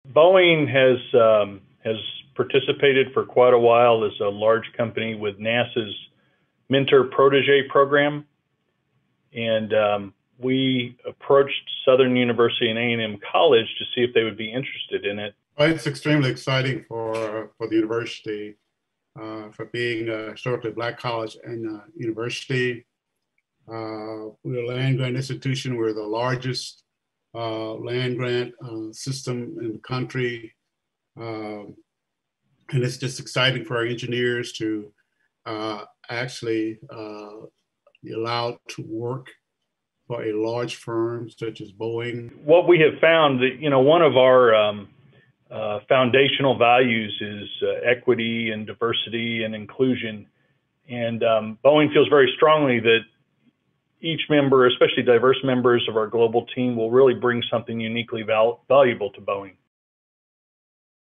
Boeing partners with historically black university
Boeing has teamed up with Southern University and A&M College to work together on NASA's Space Launch System program. Southern is a historically black university and the partnership is intended to encourage diversity in the sector. (Dec. 23)